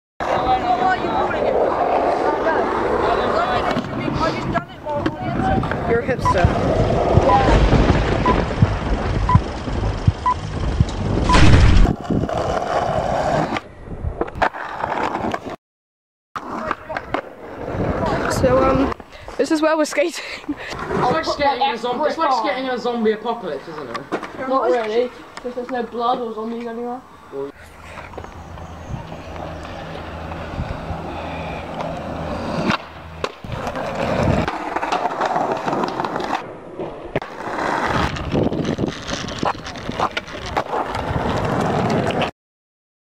Skating with logic.| WillMillarFilming.
Some shots we had in the summer and later this year. it was all a massive piss about but great fun!:D